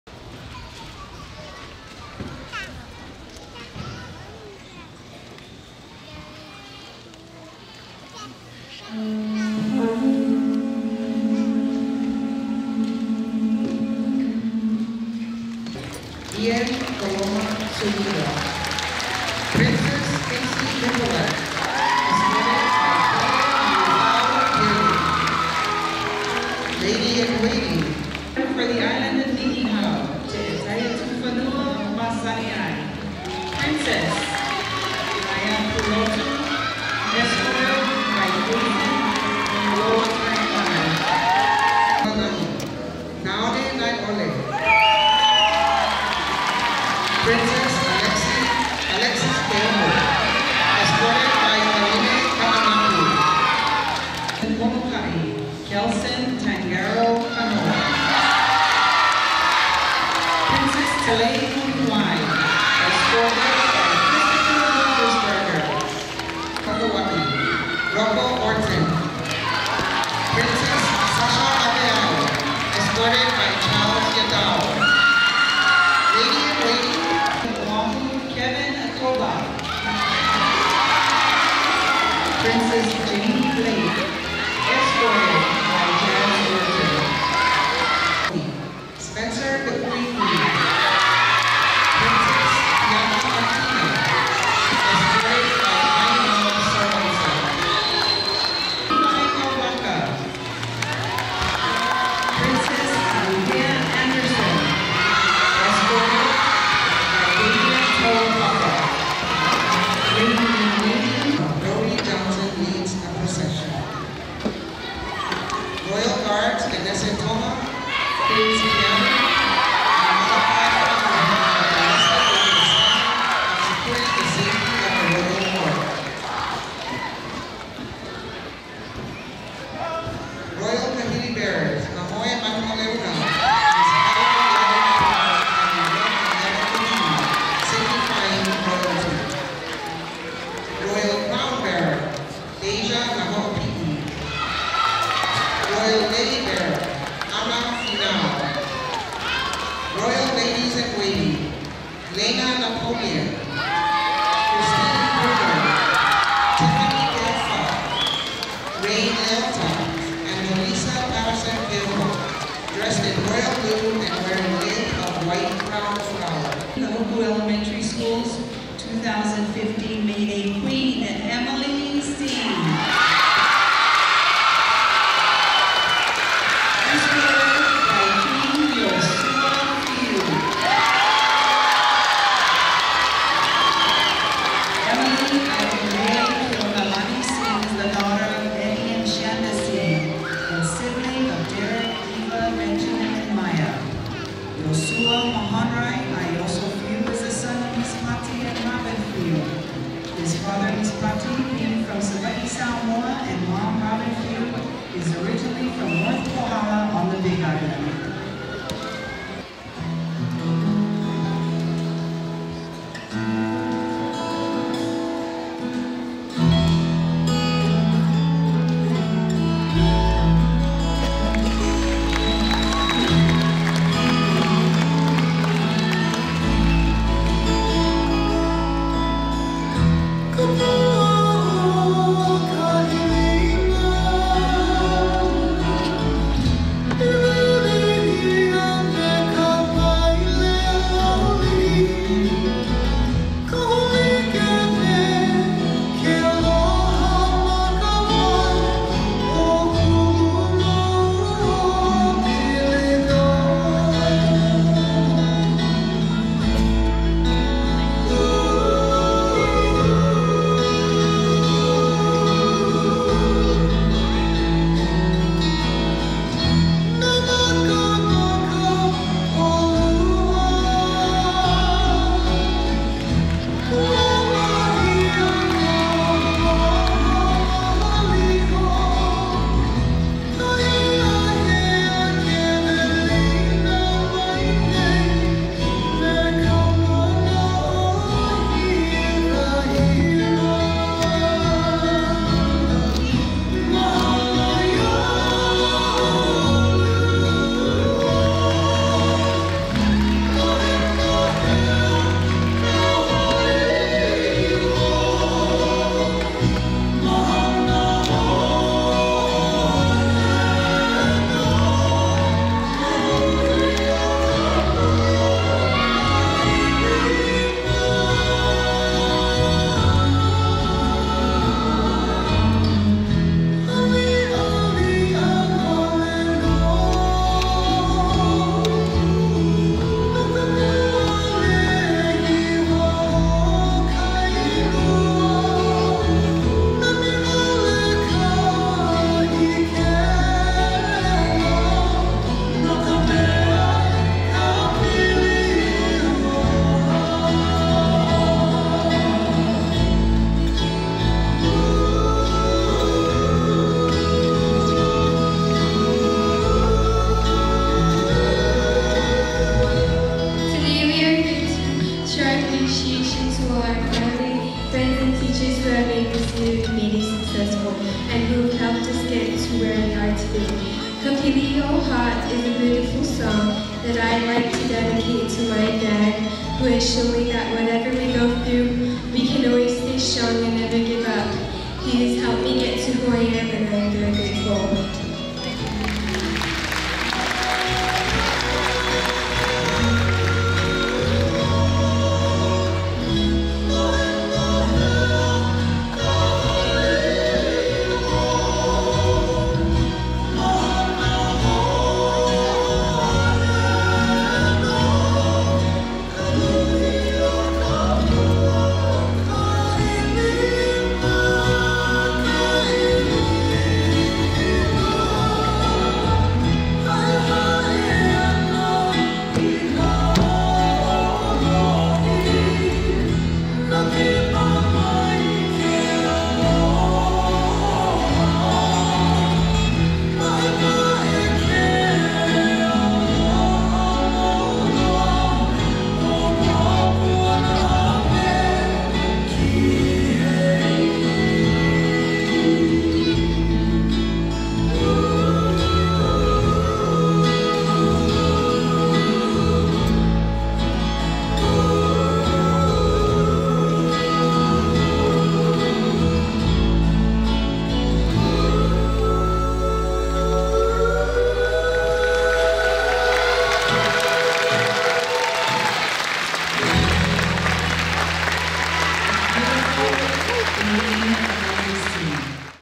Kahuku Elementary School May Day Program
The Royal Court on May Day 2015 for Kahuku Elementary School.